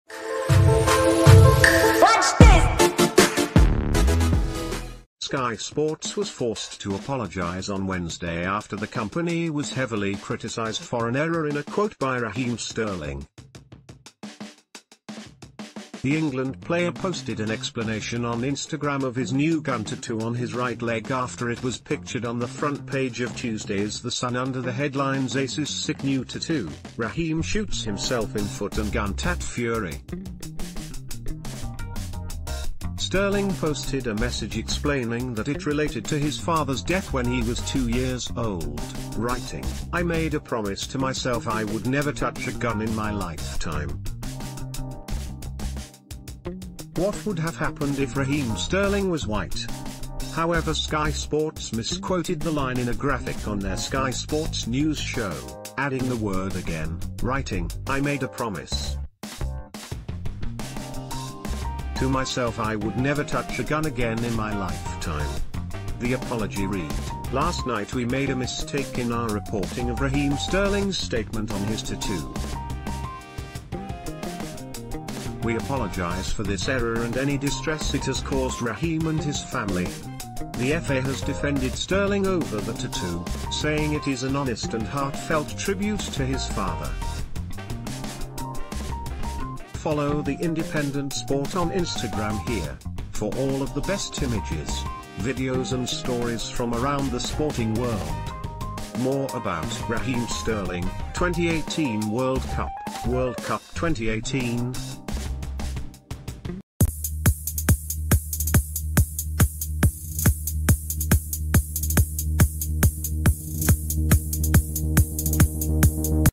Sky Sports apologized after defeating Raheem Sterling on the gunshot
Sky Sports was forced to apologise on Wednesday after the company was heavily criticised for an erro...